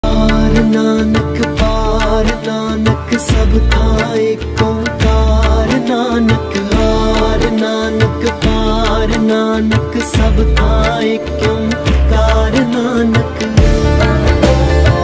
Lakhpat Gurdwara Sahib or Gurdwara Pehli Patshahi located in Lakhpat, Gujarat || Khanda
Guru Nanak on his way to Mecca stayed over in the town during his second (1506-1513) and fourth (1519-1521) missionary journey called Udasis. It is believed that he had visited the site during his fourth journey